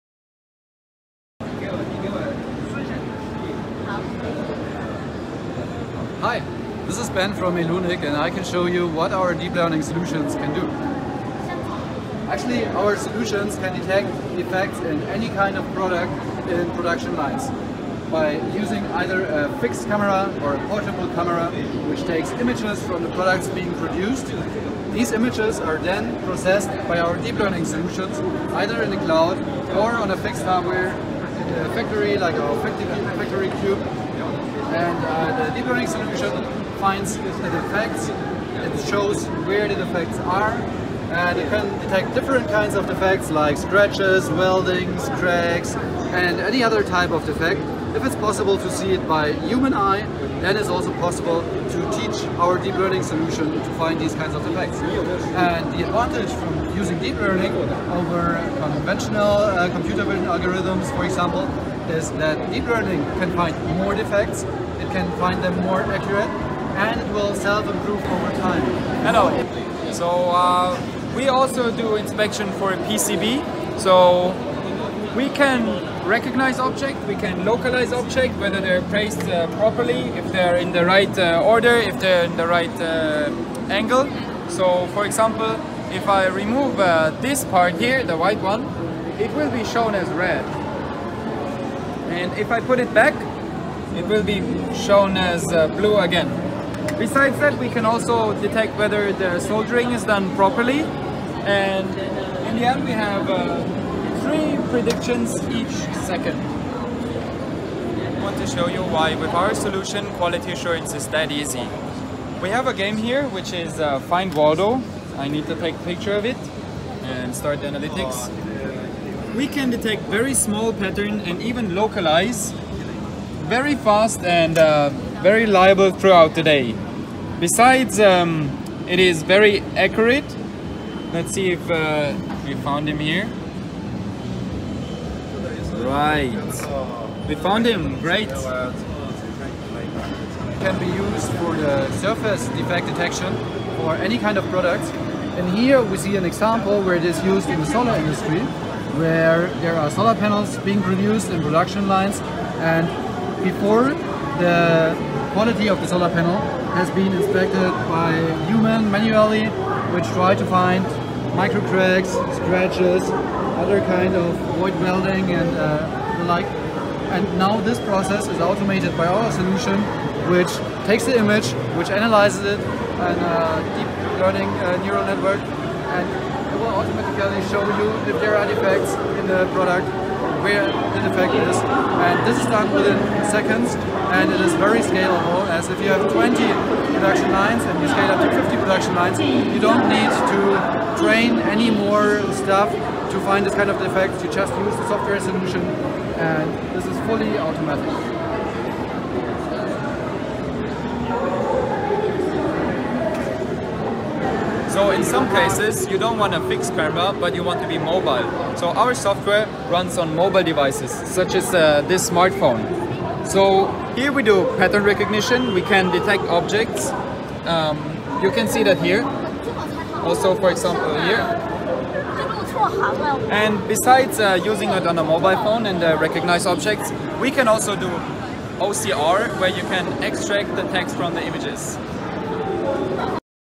elunic AI.SEE™ Showcases at International Automation Show - IAS Shanghai
AI.SEE™ – elunic’s Visual Quality Control and Defect Detection System uses next-generation Automated Optical Inspection (AOI) and self-learning models based on machine learning (ML) and deep learning (AI) algorithms capable of exploiting data potentials, even if complexity increases.